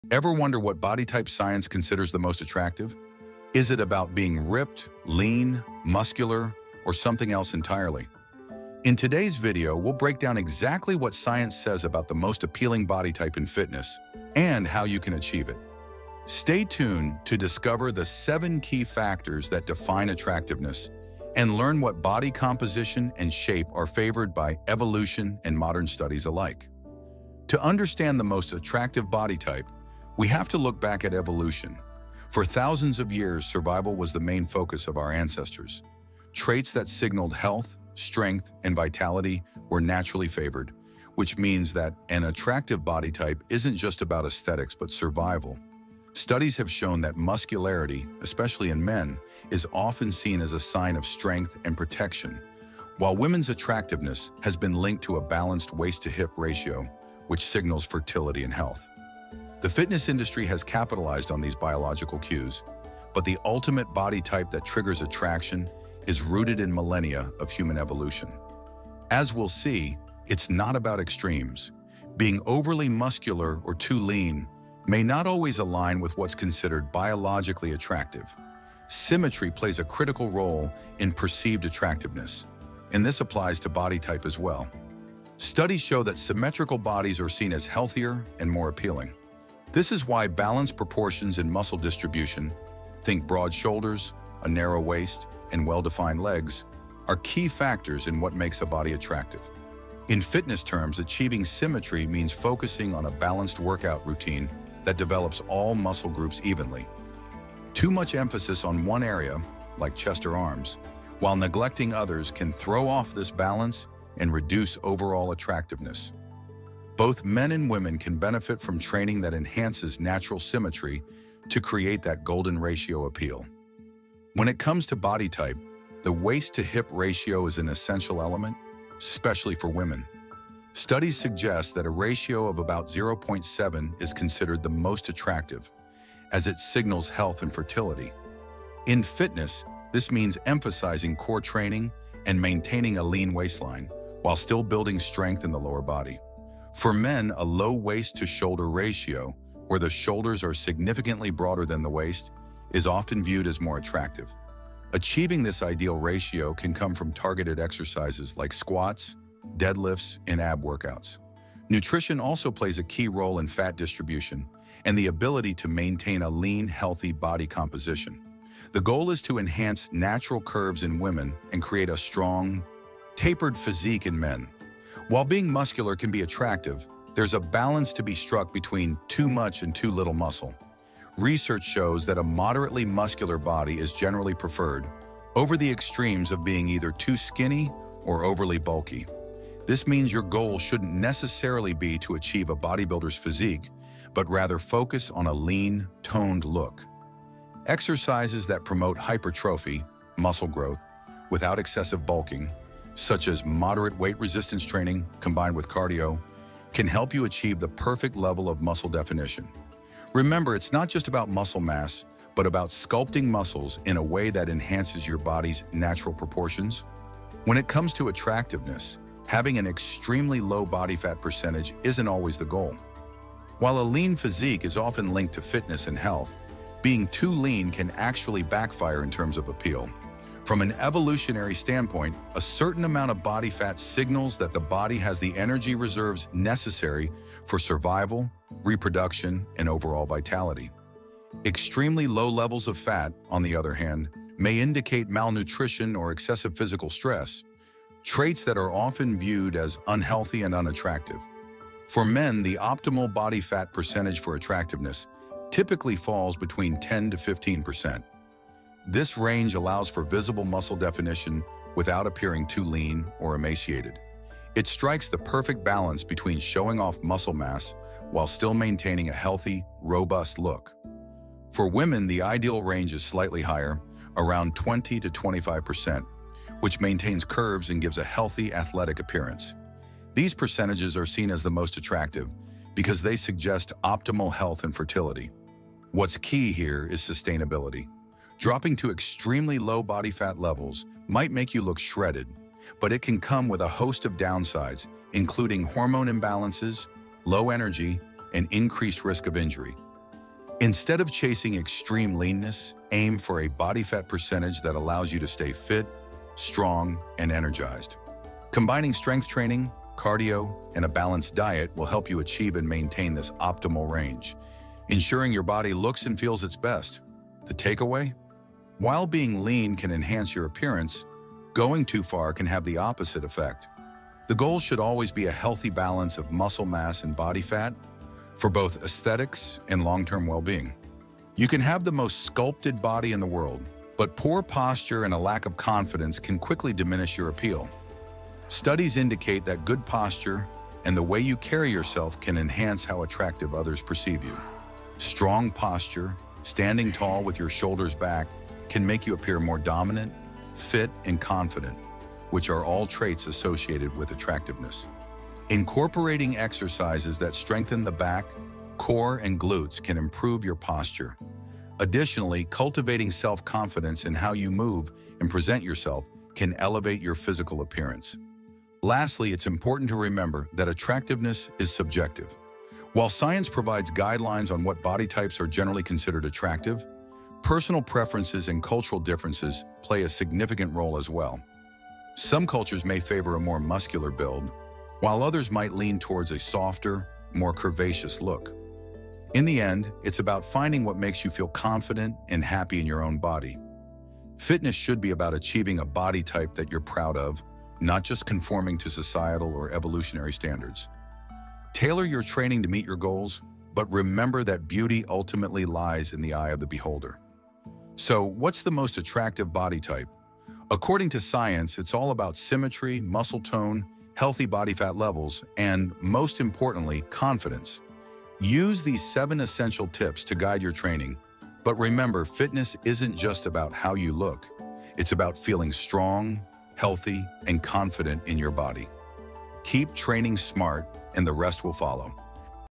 The Most Attractive Body Type (According to Science) | What Research Really Says
What does science reveal about the most attractive body type? Explore the research behind physical attractiveness, body composition, and what truly matters when it comes to fitness and appeal. Discover how you can achieve the most scientifically attractive physique.